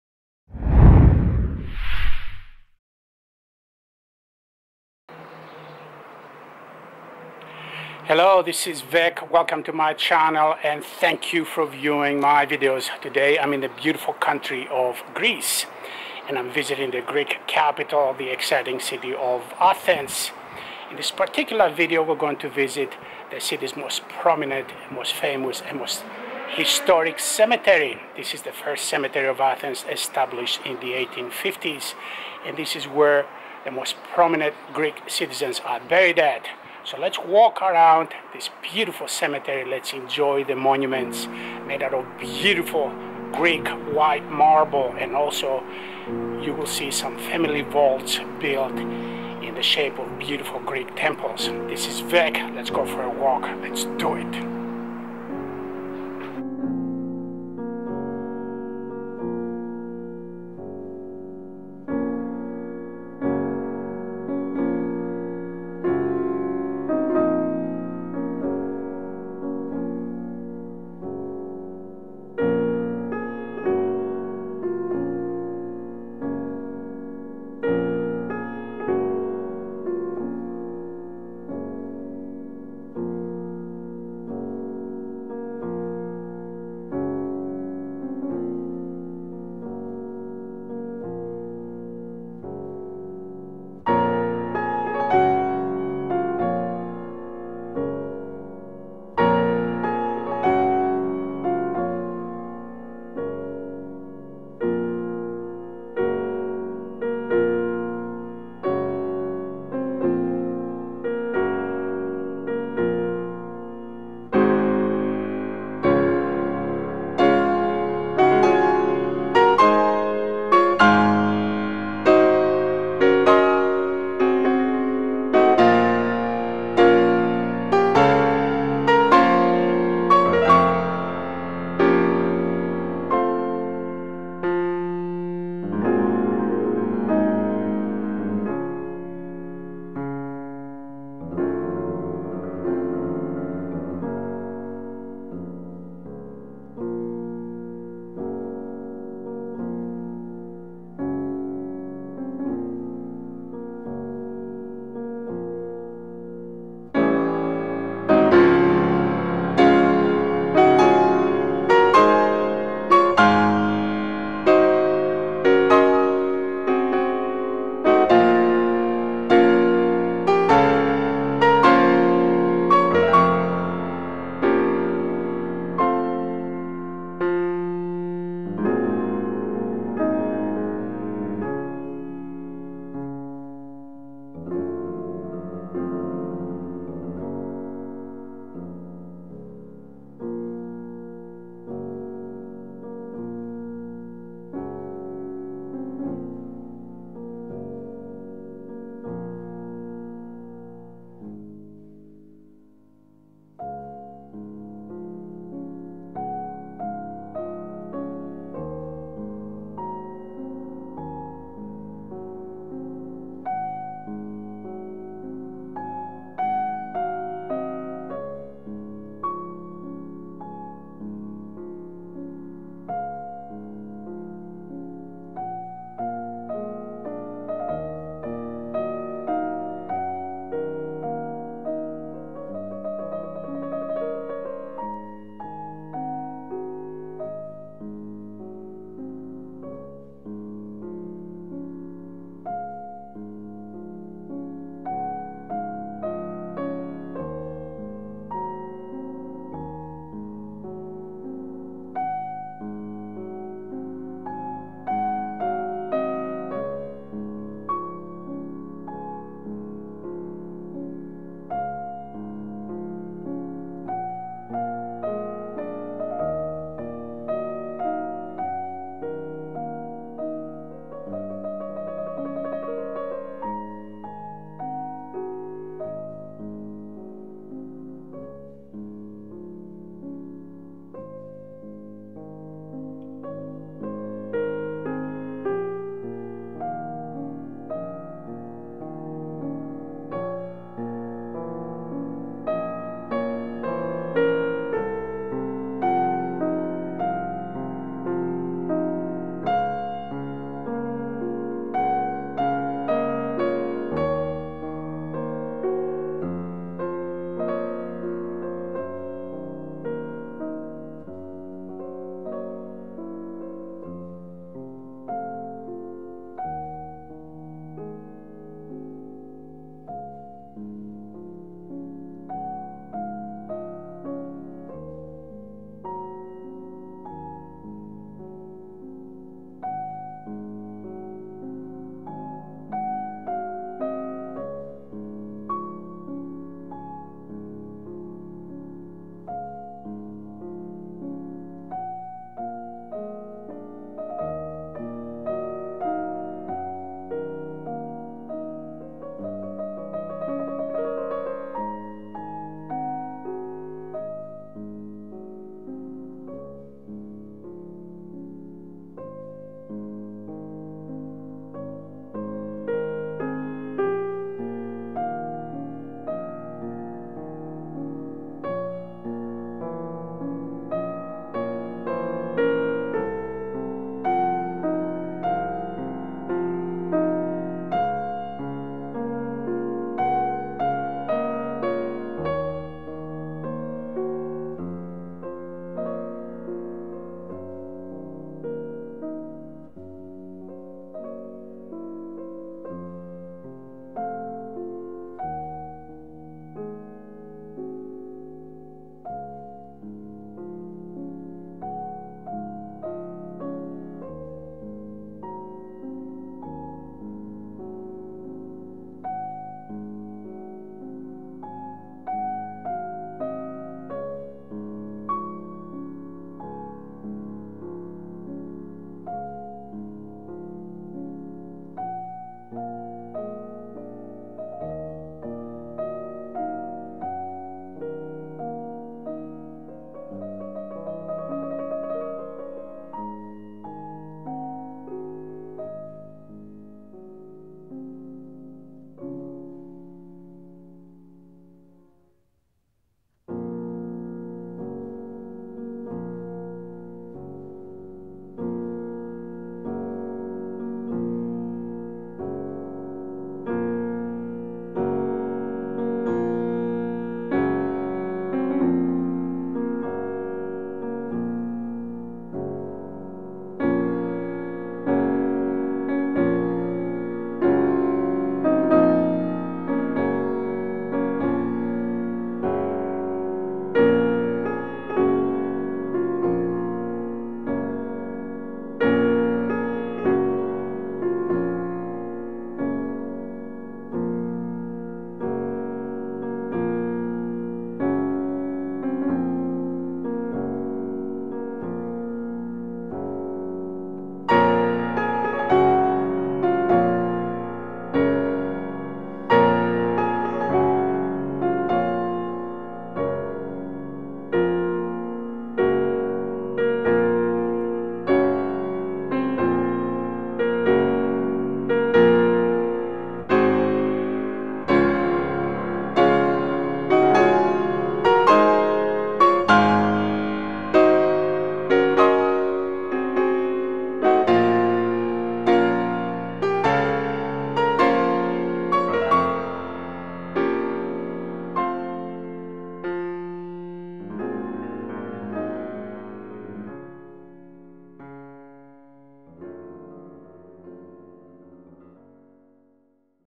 Greece travel tips: First cemetery of Athens, art & statues
Athens, Greece - First cemetery of Athens, full tour. Let's travel to Greece and let's visit the capital city of Athens and let's go to the stunning First Cemetery of Athens which is the official cemetery of the City of Athens and the first to be built. The First cemetery of Athens was opened in 1837 and soon became a prestigious cemetery for Greeks and foreigners. The first cemetery is located behind the Temple of Olympian Zeus and it is a large green space with pines and cypresses.
The first cemetery of Athens includes several impressive tombs such as those of Heinrich Schliemann, designed by Ernst Ziller; Ioannis Pesmazoglou; Georgios Averoff; and one tomb with a famous sculpture of a dead young girl called I Koimomeni ("The Sleeping Girl") and sculpted by Yannoulis Chalepas from the island of Tinos. There are also burial areas for Protestants and Jews. The cemetery has been declared an historical monument.
Athens is the capital of Greece. It was also at the heart of Ancient Greece, a powerful civilization and empire. The city is still dominated by 5th-century BC landmarks, including the Acropolis, a hilltop citadel topped with ancient buildings like the colonnaded Parthenon temple. The Acropolis Museum, along with the National Archaeological Museum, preserves sculptures, vases, jewelry and more from Ancient Greece.